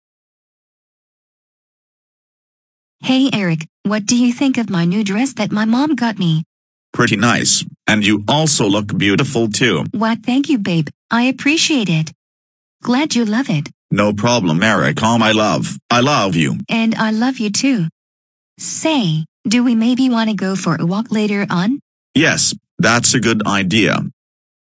Erika shows her dress to Eric
Got that done the other day, and it's here now.  I mean, I tried my best to do this.  I was just bored.  But I'll do more videos this week.